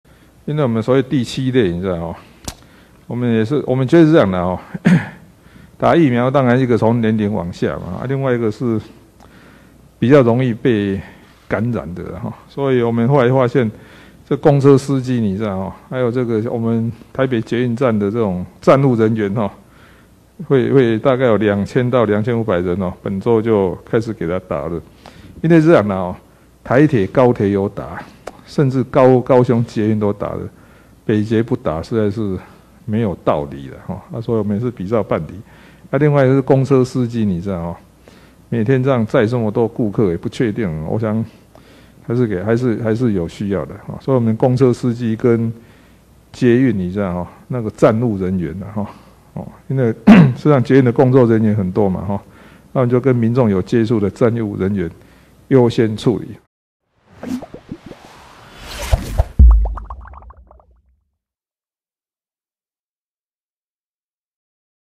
批第七類「特權疫苗」仍開放北捷施打 柯文哲：高捷都打了 不打沒道理｜中時新聞網
台北市第五波疫苗明起開始預約，台北市長柯文哲宣布，7月6、7日兩天將開放給70歲以上與55歲以上的原住民預約，並在7月8日至10日開始接種，而這次的預約系統也新增「預約取消」功能，柯文哲呼籲，不想打就取消，別浪費名額，不然疫苗準備在那裡很麻煩。

【相關新聞】
北市6日起開放70歲長者預約疫苗　北捷、公車員工今明接種